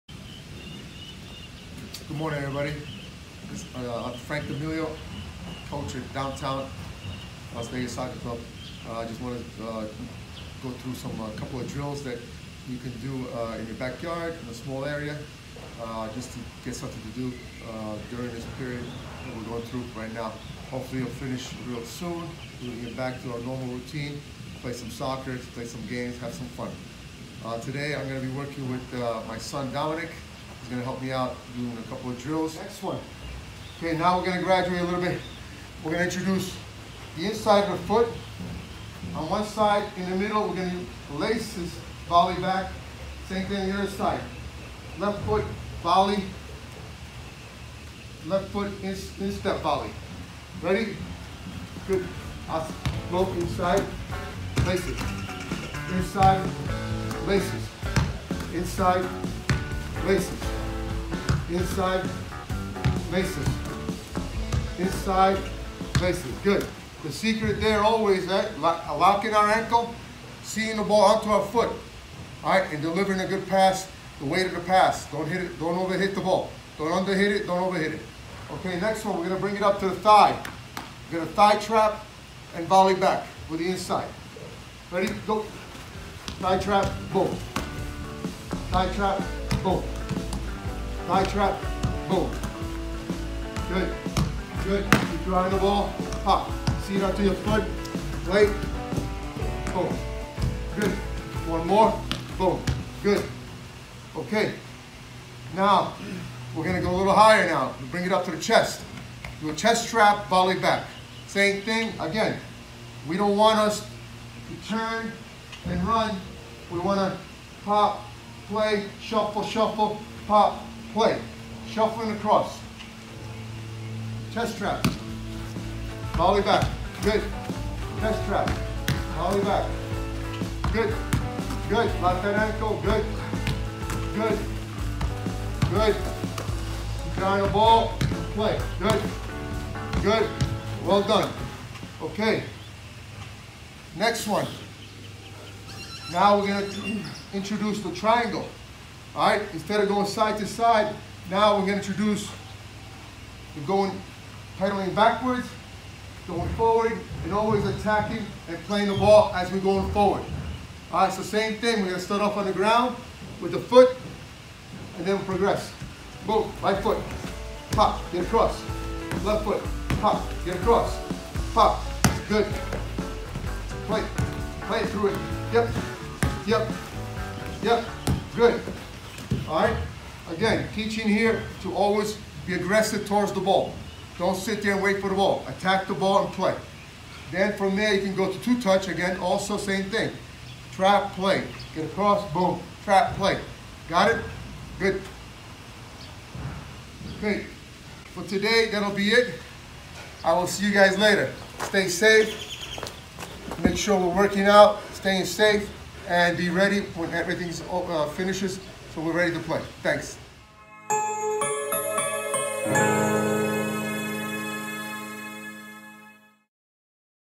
Coach Frank Training From Home 2
DLVSC coach Frank D'Amelio & his son Dominic are training everyone from home. They are demonstrating drills you can do on your own or with help from a parent or sibling. Be safe, stay home, practice social distancing.